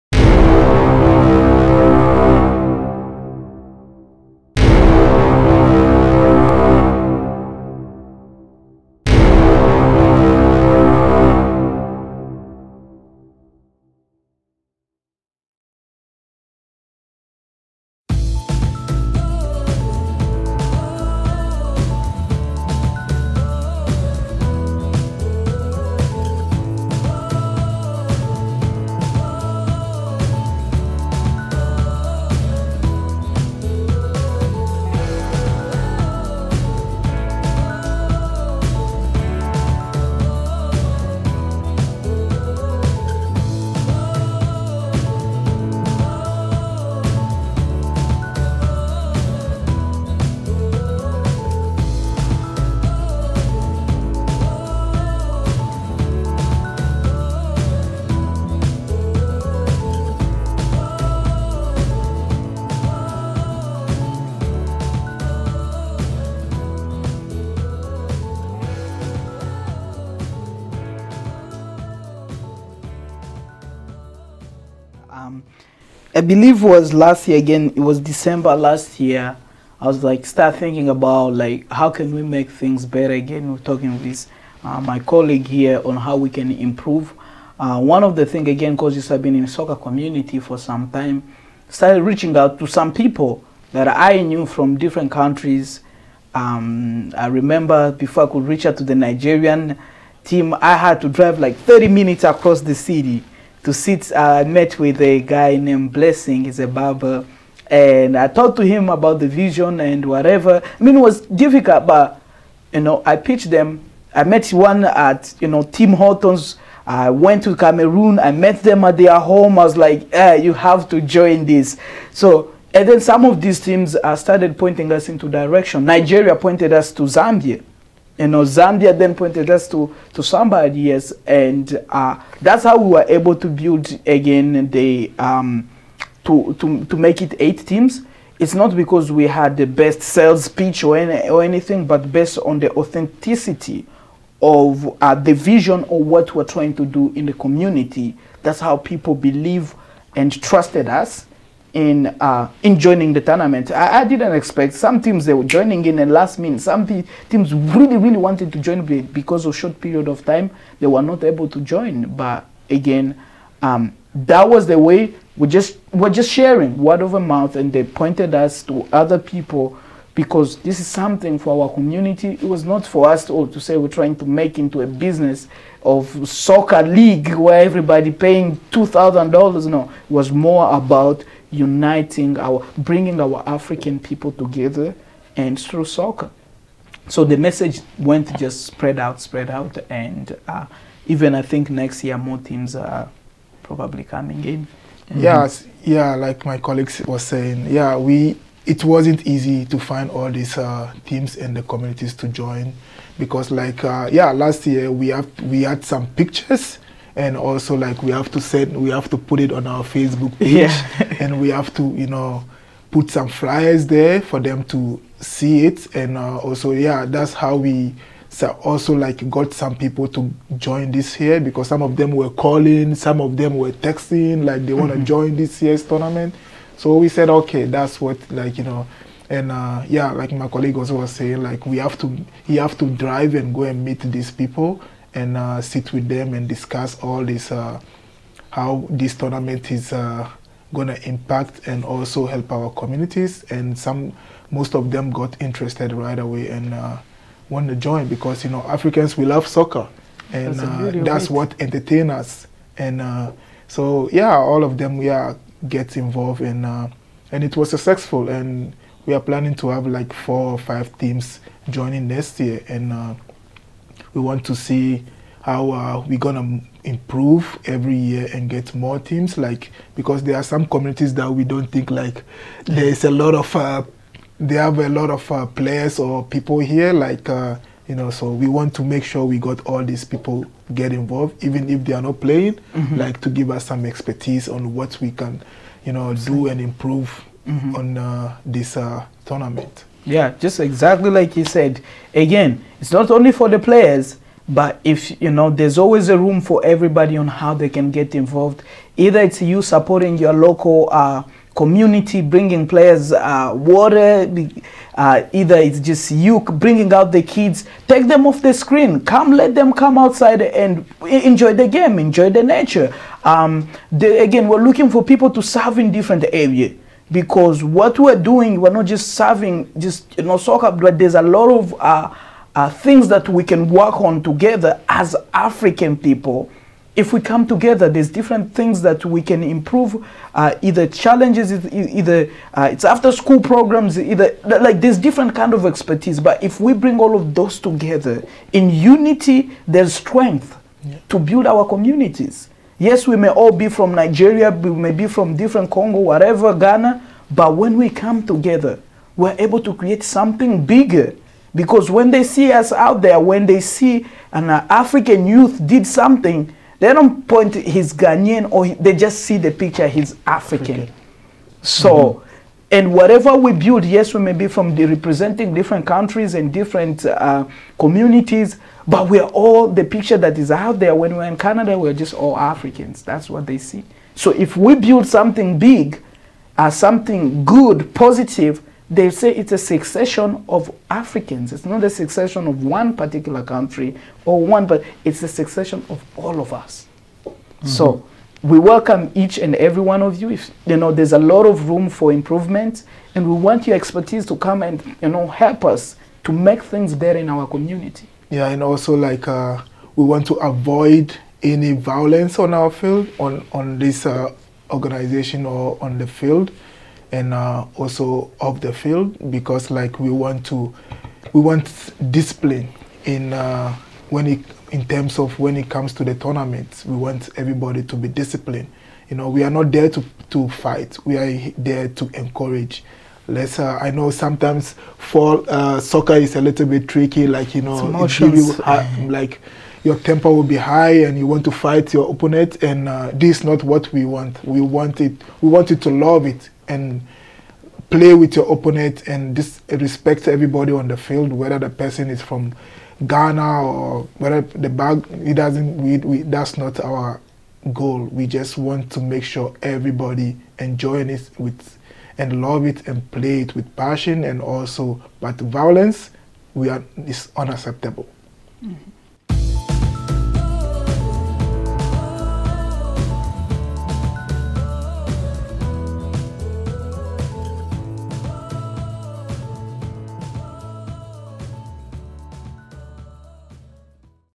Part 2/5 - How MACN get 4 more countries (Teams) to join the tournament 2020
Inside the MACN and African Communities in Canada - WSA